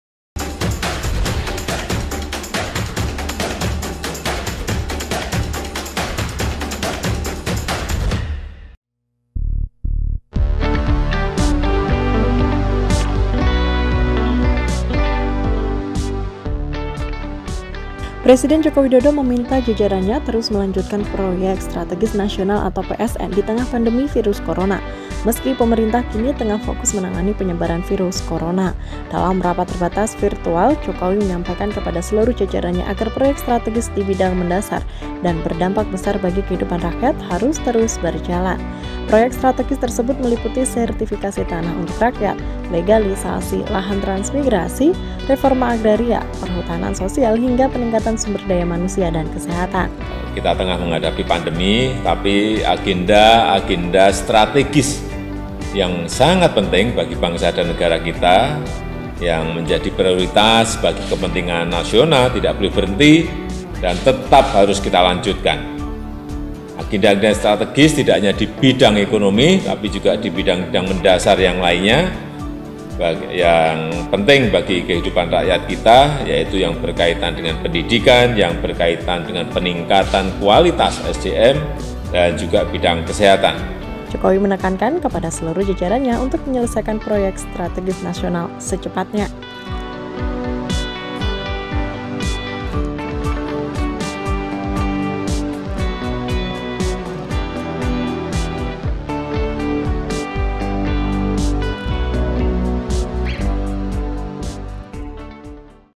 Jokowi: Proyek Strategis Nasional Harus Tetap Dikebut
Presiden Joko Widodo meminta jajarannya terus melanjutkan proyek strategis nasional (PSN) di tengah pandemi virus Corona.
Terutama proyek-proyek strategis di bidang mendasar dan berdampak besar bagi kehidupan rakyat harus terus berjalan.